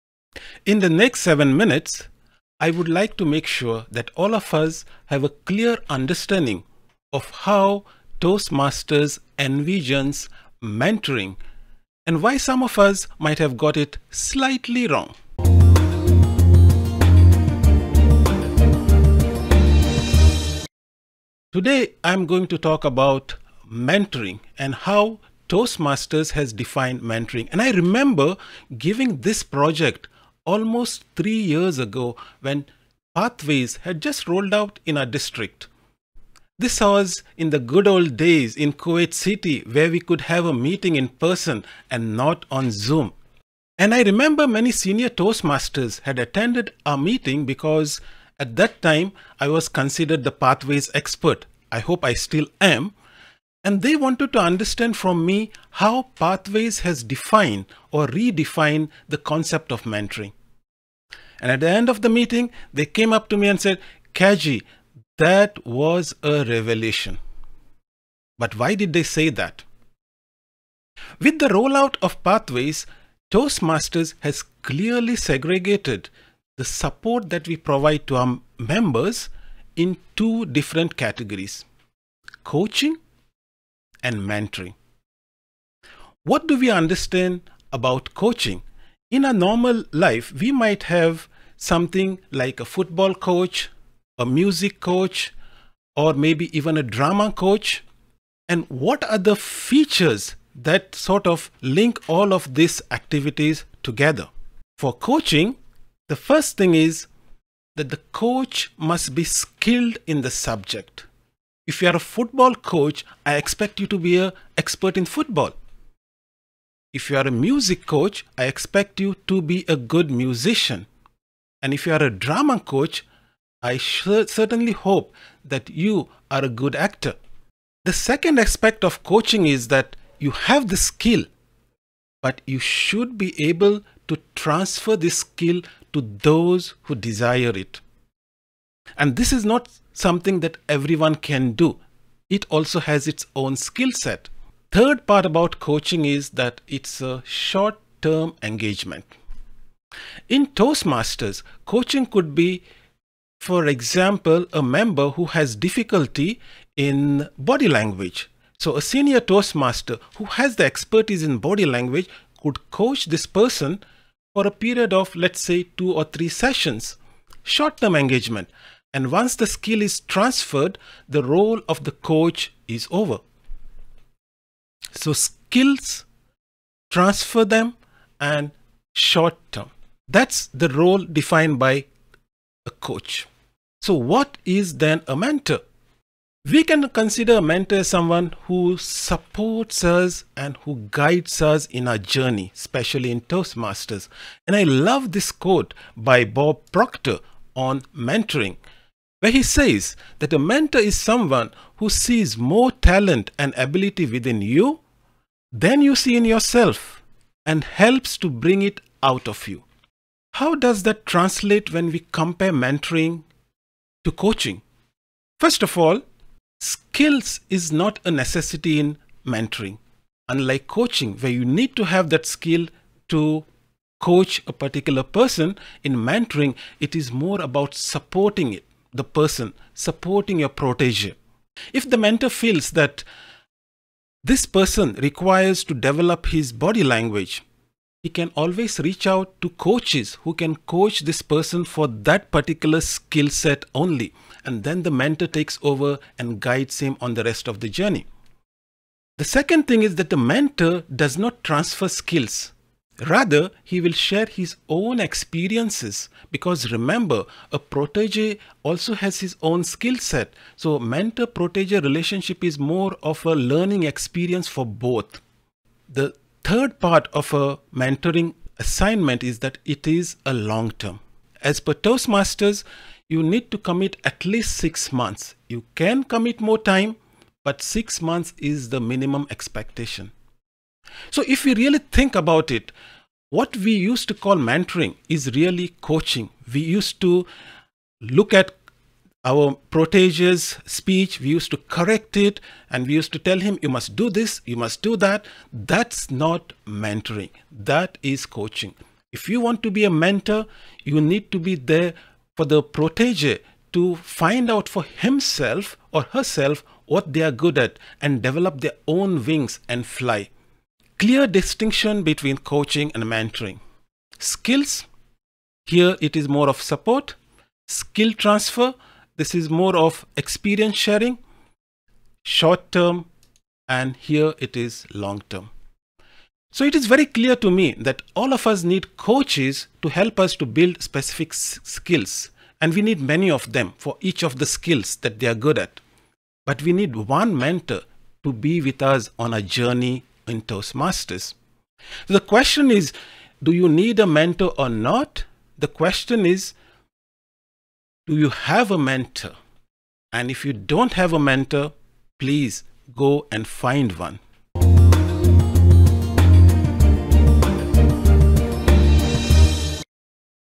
Mentoring vs Coaching in Toastmasters : What's the difference?
Toastmasters envisions mentoring as a long term activity (at least 6 months commitment) while coaching is defined as a short term activity to achieve specific goals of the member.